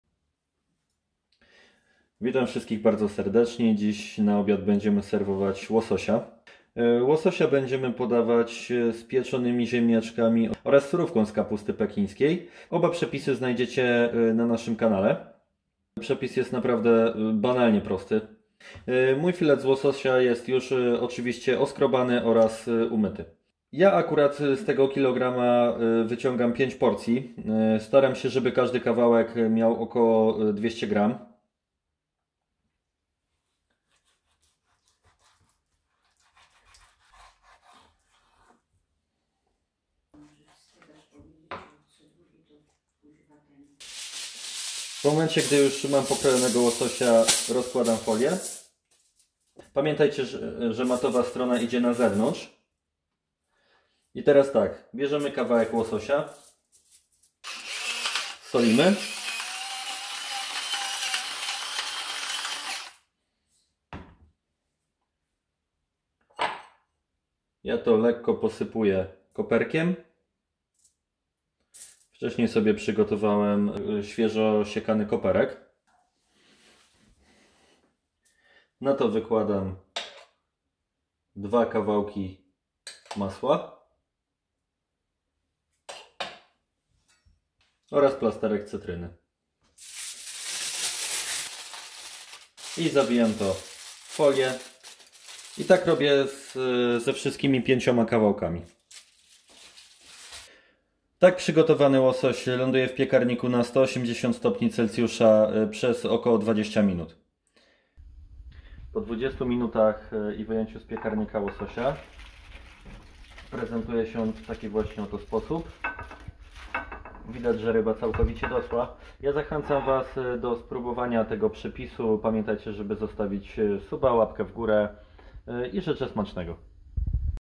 🍋 Pieczony łosoś z masłem i cytryną 👍 | Soczysty łosoś z piekarnika / Nasze dania
Zastanawiacie się jak nie wysuszyć ryby, już nie musicie, nasz szybki i prosty przepis na łososia z piekarnika rozwieje wasze wątpliwości. Potrzebujesz dosłownie 20 minut aby przyrządzić pyszną soczystą rybę w folii. Filety z łososia pieczone z masłem i cytryną w piekarniku przemieniają się w rybę w maślanym sosie aromatyzowanym cytryna i koperkiem. Filet z piekarnika to zdrowy obiad lub pożywna kolacja.

Składniki (na 5 porcji):
1 kg fileta z łososia
1 cytryna
40g masła
pęczek koperku
sól

Jeśli skorzystasz z naszego przepisu, koniecznie oznacz nas na instagramie lub podziel się wrażeniami w komentarzach :)